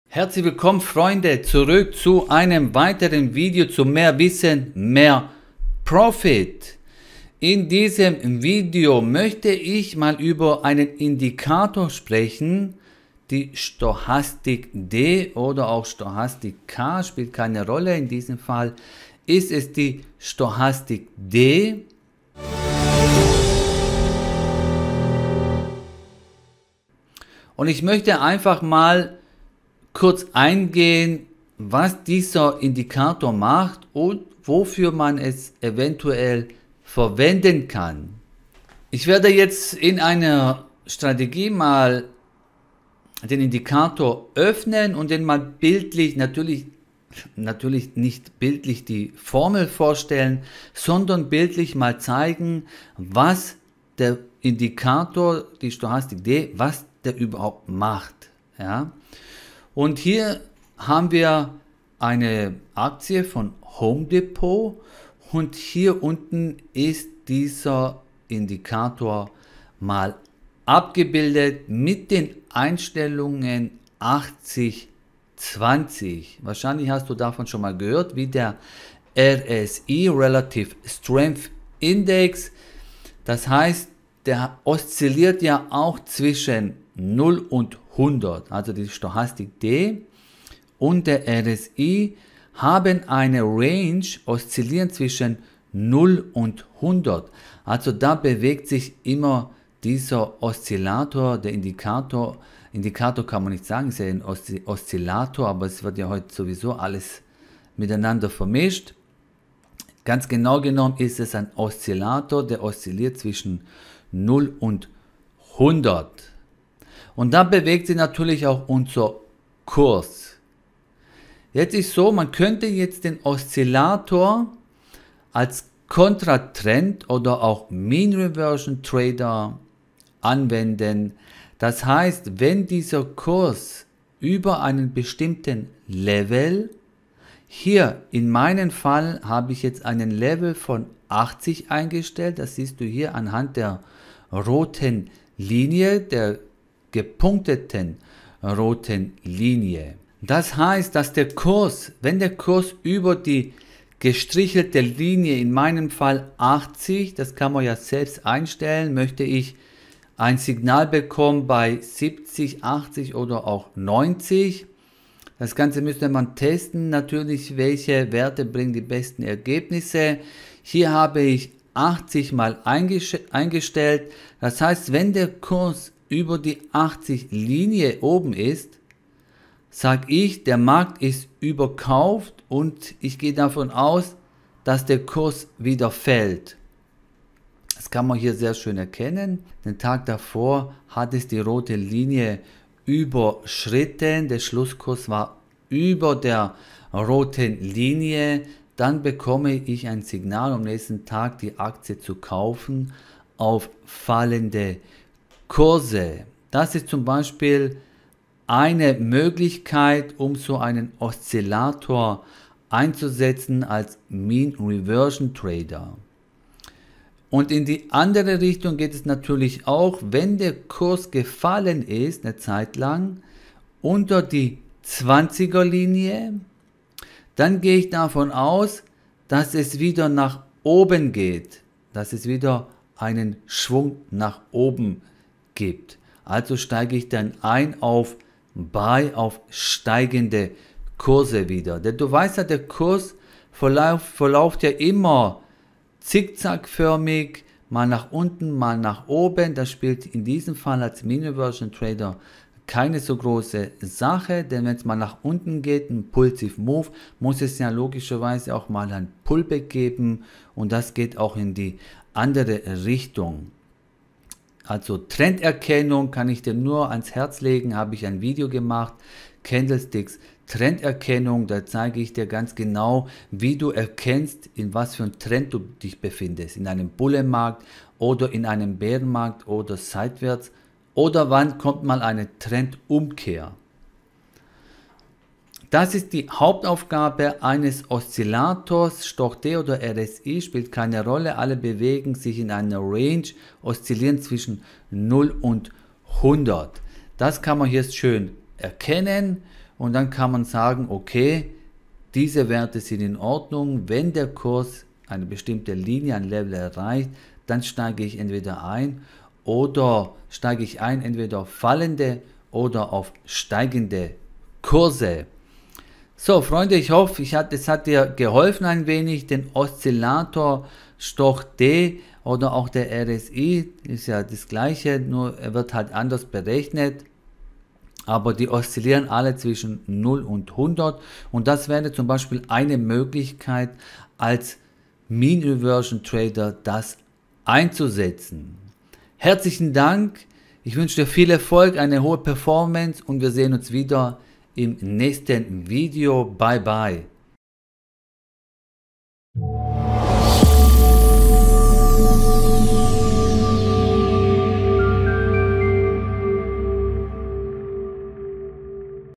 So hast du den Indikator, die Stochastik, noch nie gesehen
00:00 Intro Indikator Stochastik
00:27 Anwendung und Funktionsweise
02:55 Funktionsweise am Chart
05:39 Outro und Videoempfehlung

Fragen oder Anregungen?